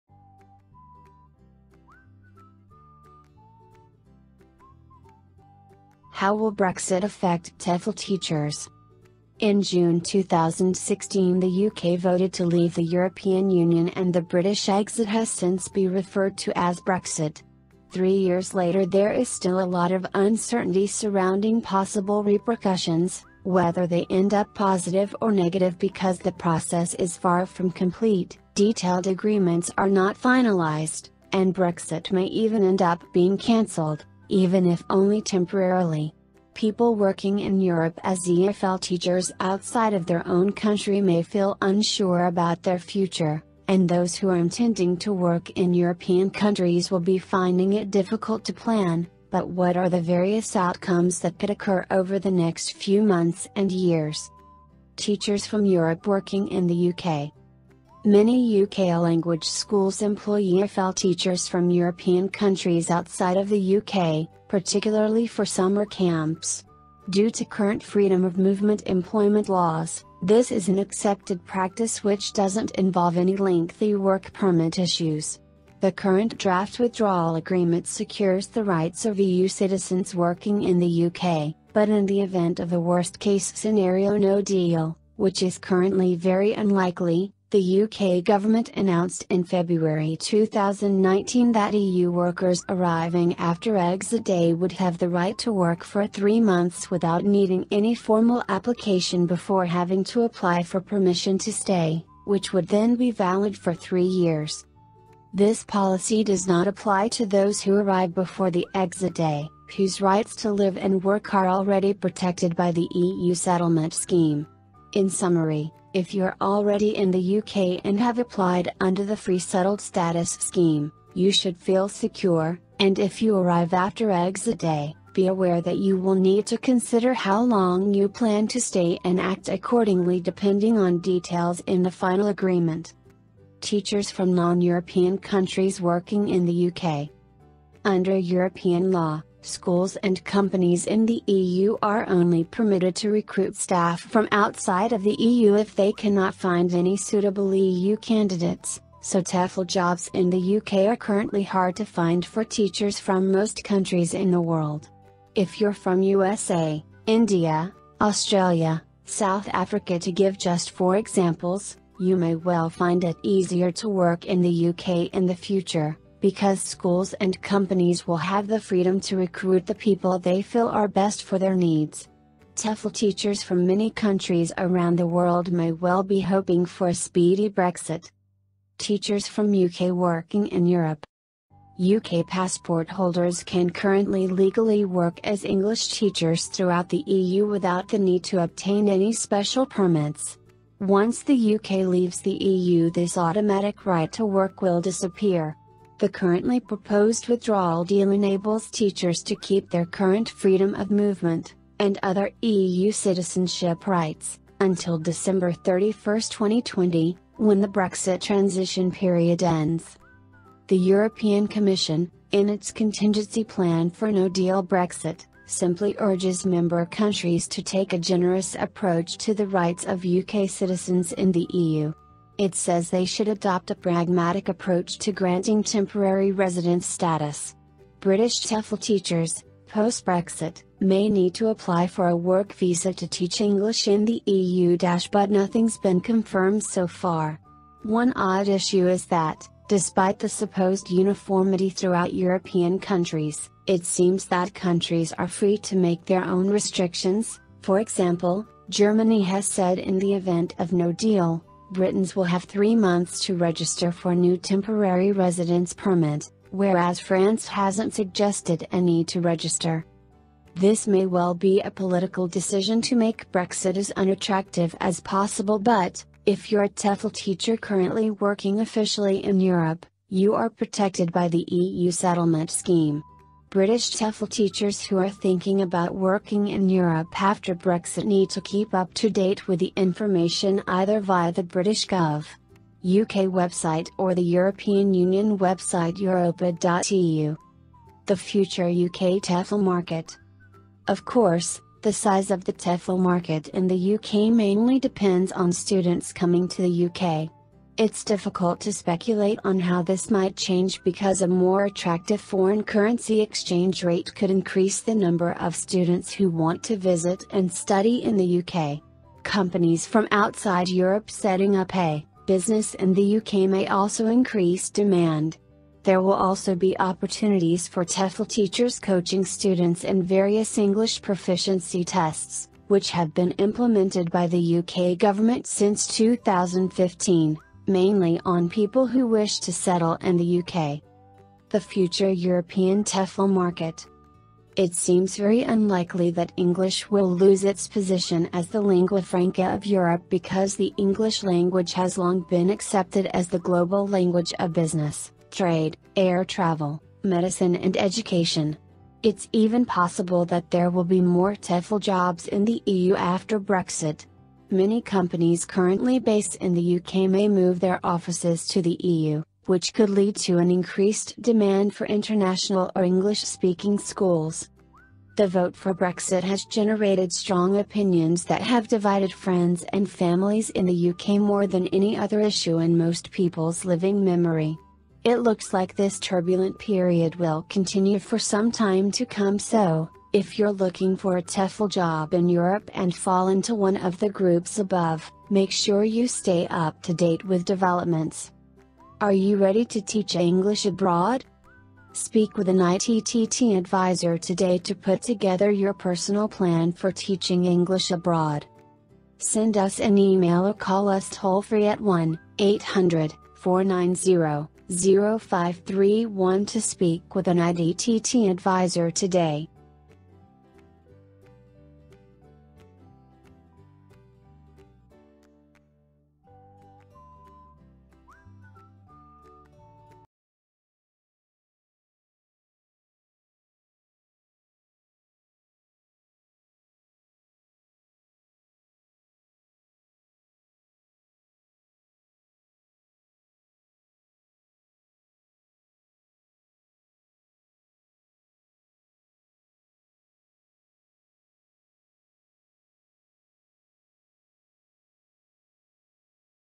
How Will Brexit Affect TEFL Teachers | ITTT TEFL BLOG
In June 2016 the UK voted to leave the European Union and the “British exit” has since be referred to as “Brexit”. Three years later there is still a lot of uncertainty surrounding possible repercussions, whether they end up positive or negative because the process is far from complete, detailed agreements are not finalized, and Brexit may even end up being canceled – even if only temporarily. People working in Europe as EFL teachers outside of their own country may feel unsure about their future, and those who are intending to work in European countries will be finding it difficult to plan, but what are the various outcomes that could occur over the next few months and years?